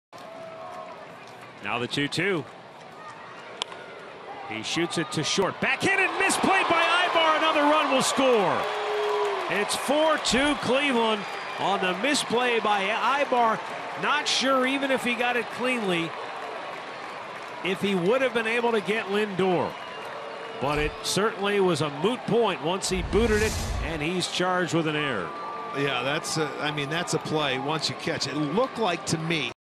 CLE@ATL: Davis crosses the dish on Aybar's error
Francisco Lindor slaps a ball to short that Erick Aybar misplays, allowing Rajai Davis to cross the plate for an insurance run in the 9th